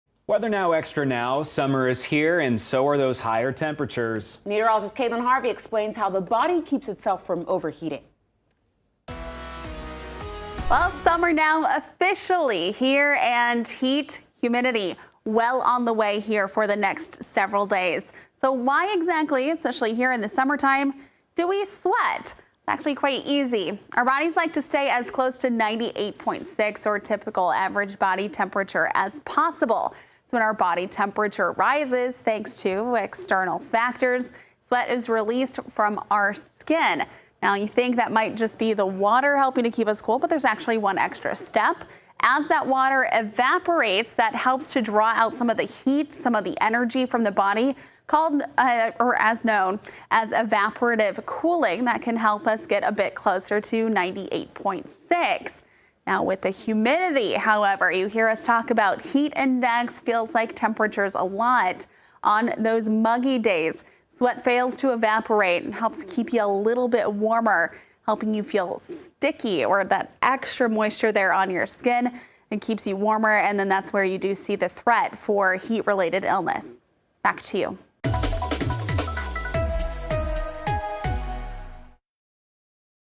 KETV meteorologist explains why exactly, especially here in the summertime, do people sweat?
Summer is here and so are higher temperatures. With that in mind, KETV meteorologist Caitlin Harvey explained how the body keeps itself from overheating.